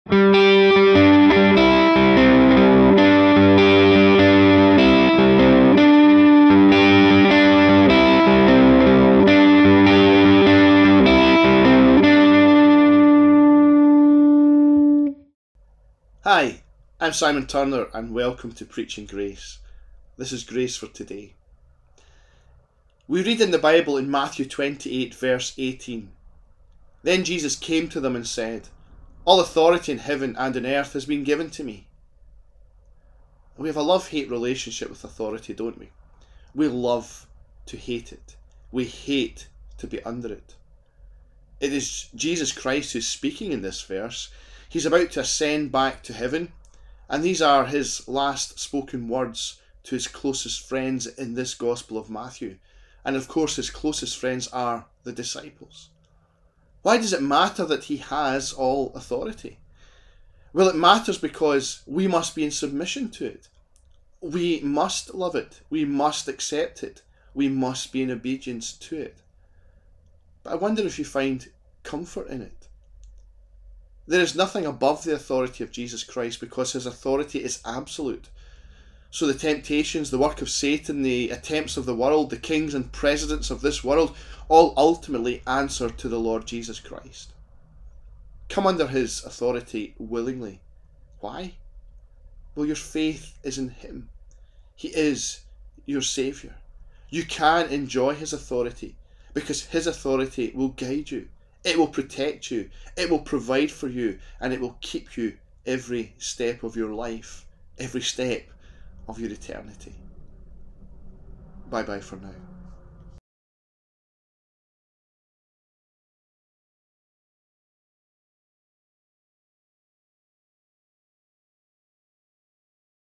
Loving The Authority Of Jesus
Our devotion this week by Simon Turner from Matthew 28:18.